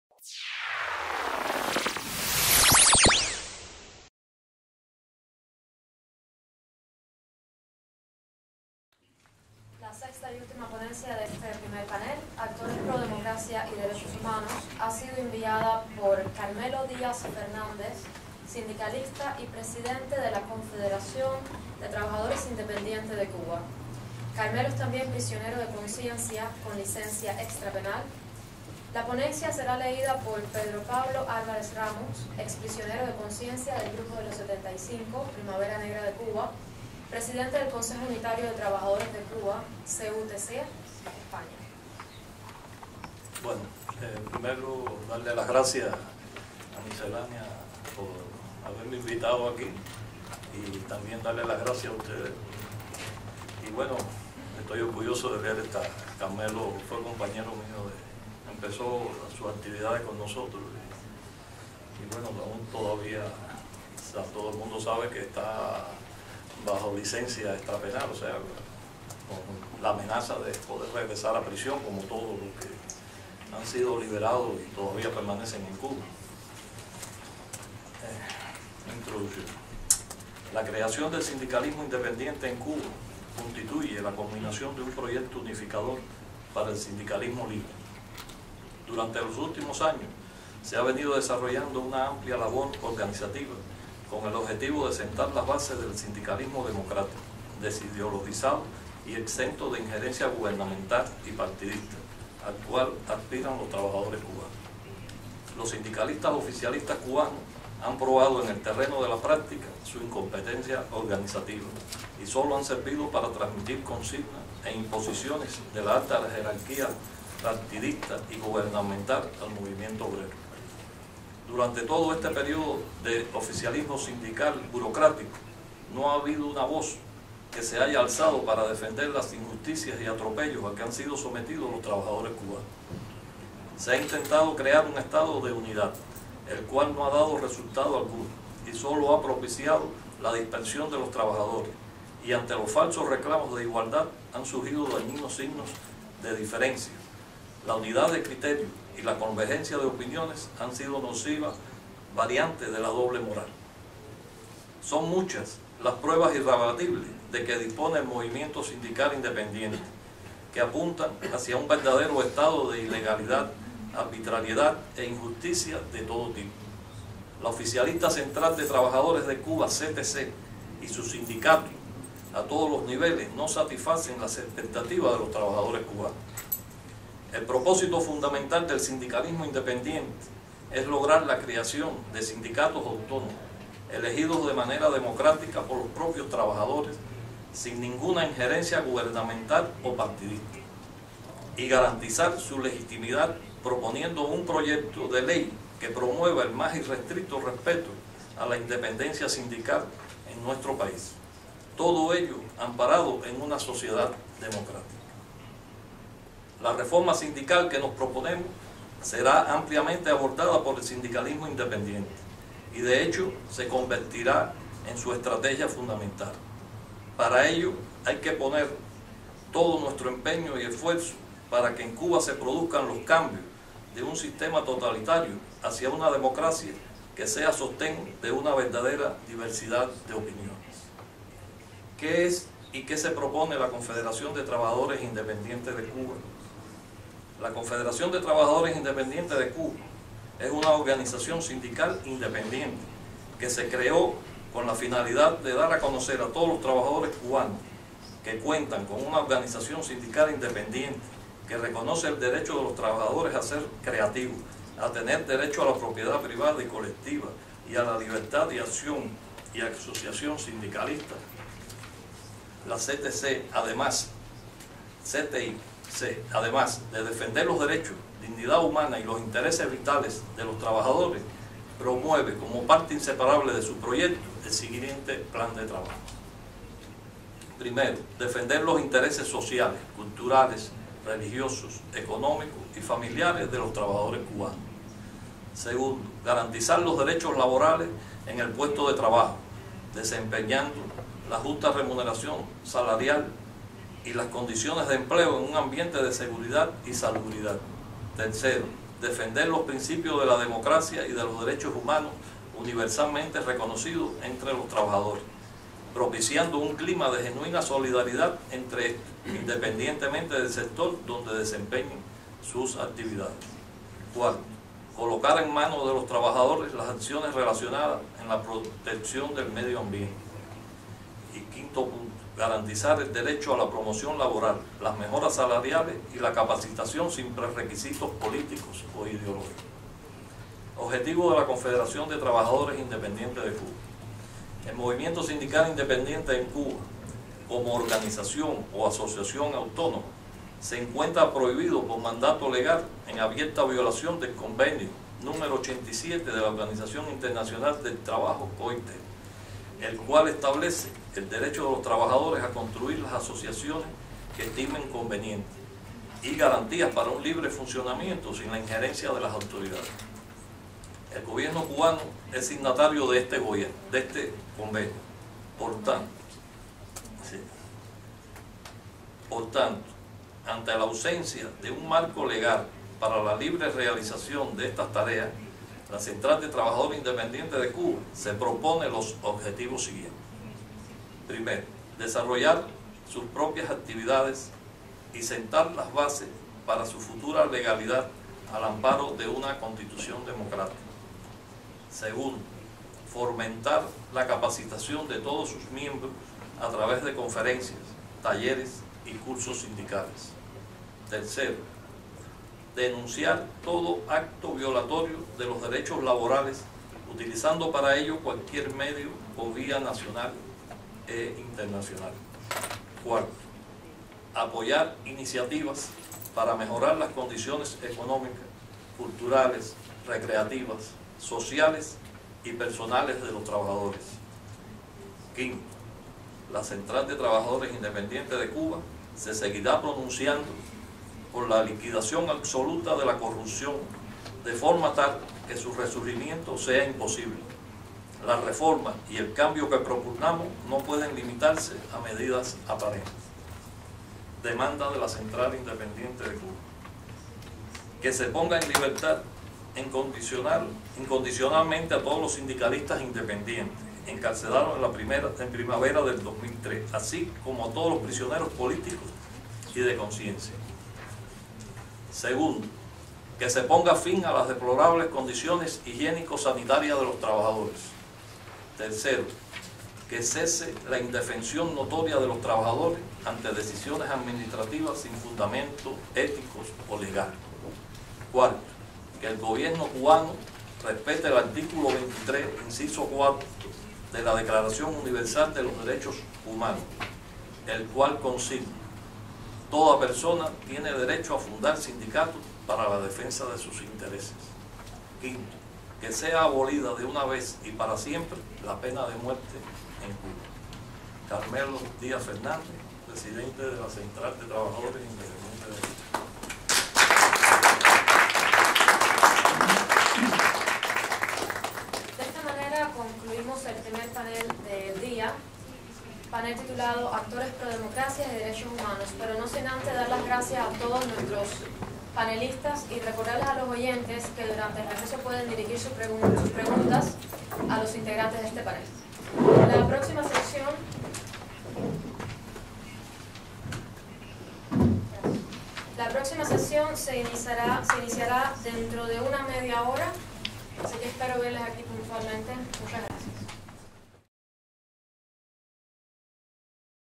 Pedro Pablo - Panel I - Seminario Internacional - 5to Aniversario Misceláneas de Cuba
Pedro Pablo, ex-preso de conciencia de la Primavera Negra de 2003 y secretario general del CUTC, hizo lectura de la ponencia enviada por Carmelo Díaz Fernández, Confederación de Trabajadores Independientes de Cuba, en el primer panel del Seminario Internacional "Los actores pro democracia en la sociedad civil y la situación de los derechos humanos en Cuba" celebrado en el Hotel Clarion, Estocolmo, Suecia, el pasado10 de Septiembre de 2009. El evento fue patrocinado por el Centro Internacional Liberal Sueco (Swedish International Liberal Centre, SILC)